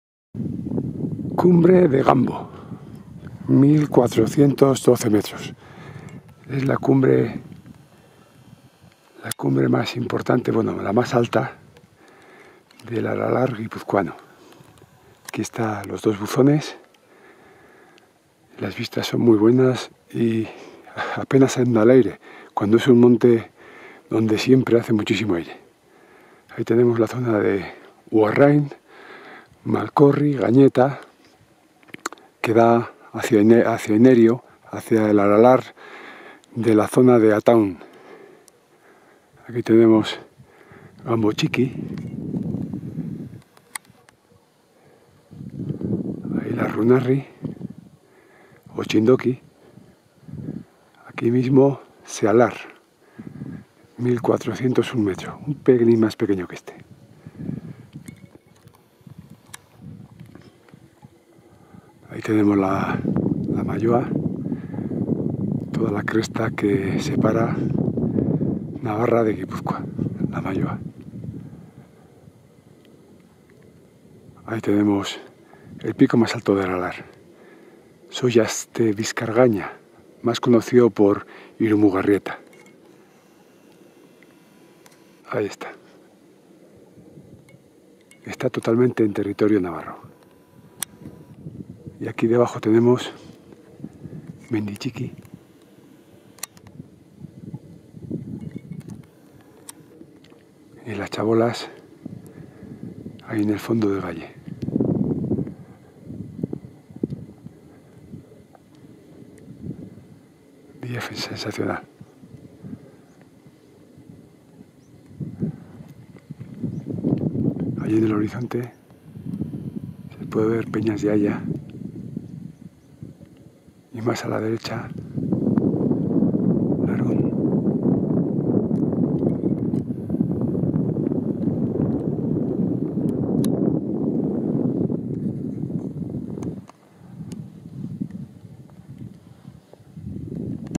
Ganboa 1 413 m Aralar Guipúzcoa
Se trata de la cumbre más elevada del aralar Guipuzcoano. En este caso el ascenso ha sido realizado por Amézketa pero igualmente se asciende por todas las vertientes y sin dificultad. La lejanía es lo más destacable.